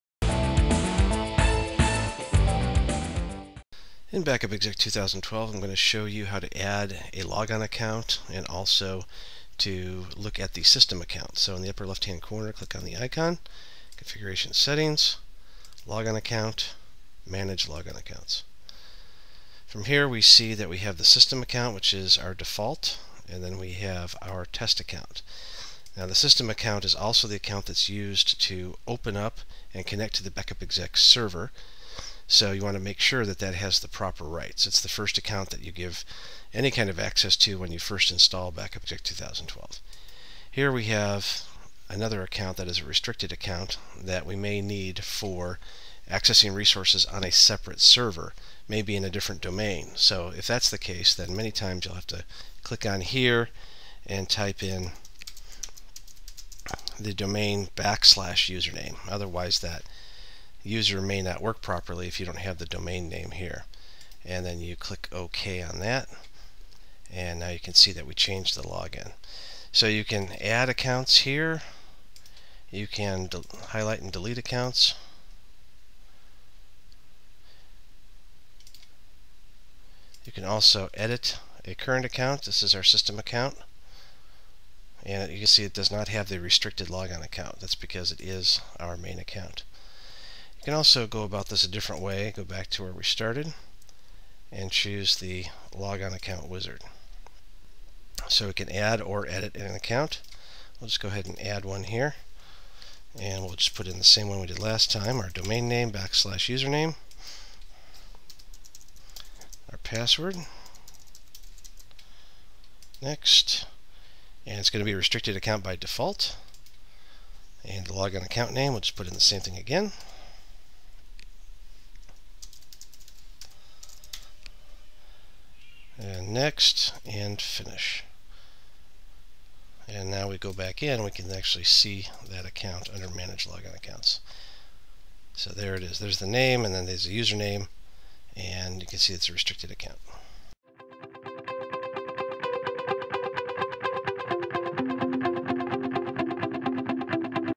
How to create and manage logon accounts for Backup Exec 2012
Author and talk show host Robert McMillen explains how to create and manage logon accounts for Backup Exec 2012.